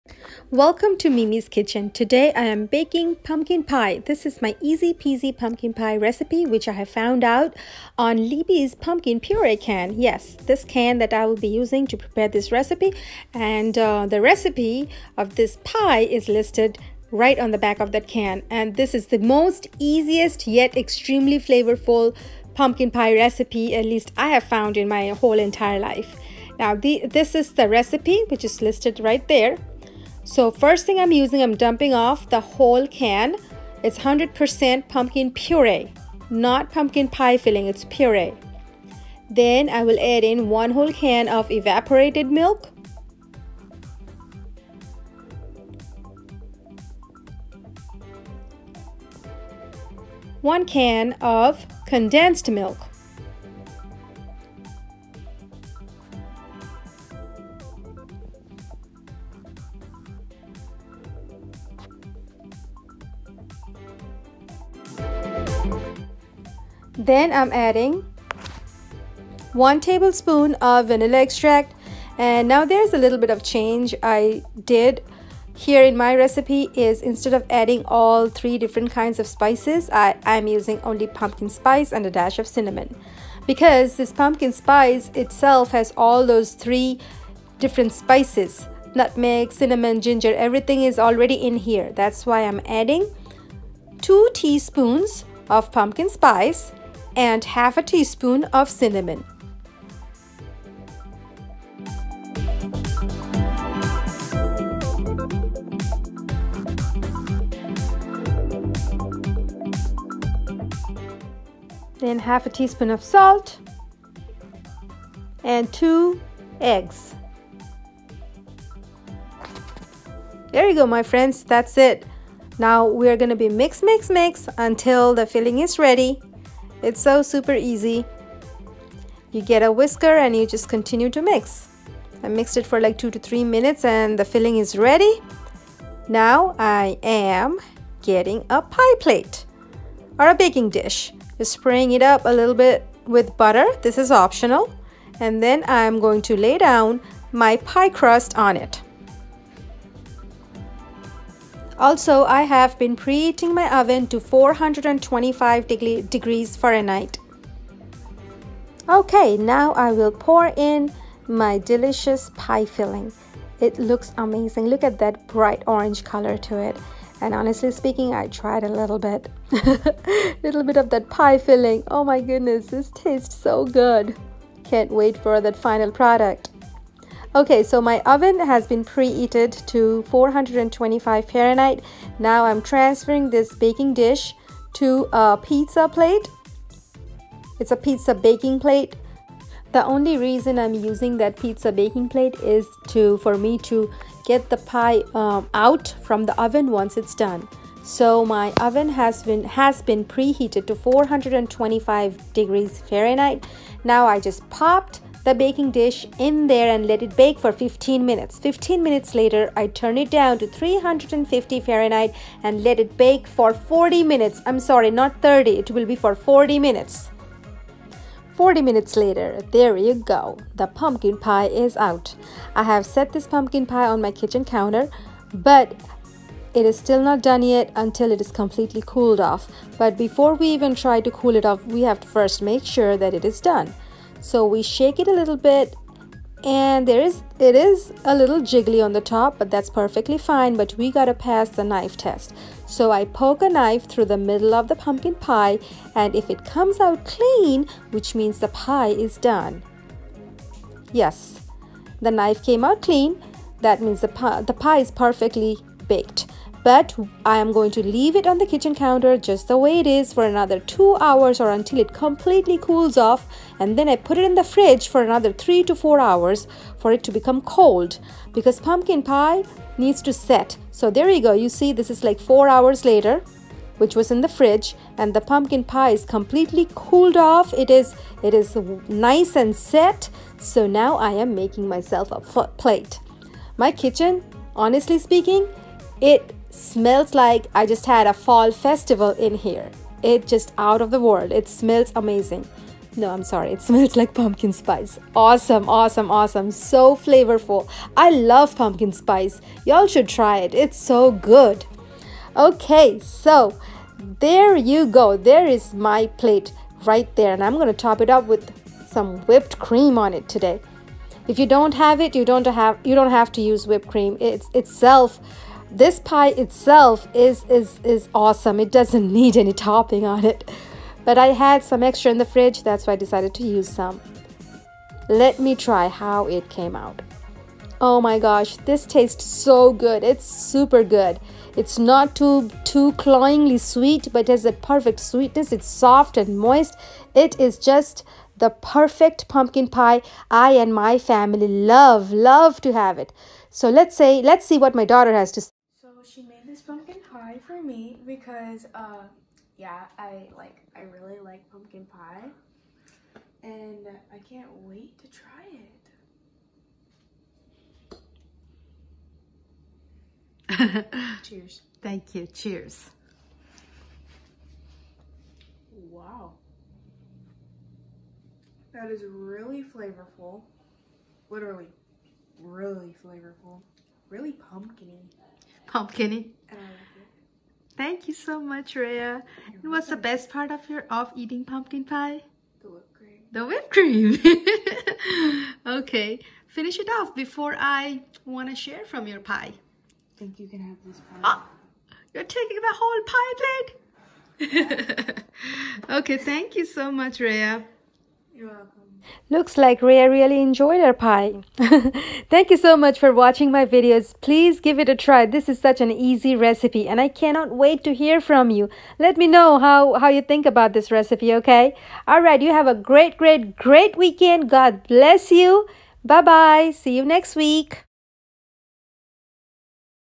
Delicious Pumpkin Pie
pie crust
1 can Pumpkin puree
1 can evaporated milk
1 can condensed milk
2 tsps pumpkin spice
1/2 tsp cinnamon
1/2 tsp salt
2 large eggs
1tbsp vanilla extract

Baking timing will be 425F for 15 mins and the turn it down to 350F for 40 mins.
after the pie is done, make sure to cool it off completely on the kitchen counter before putting it to the fridge
let it refrigerate for atleast 2-3 hr for the pie to set and cool off nicely.
topping with whipped cream is optional.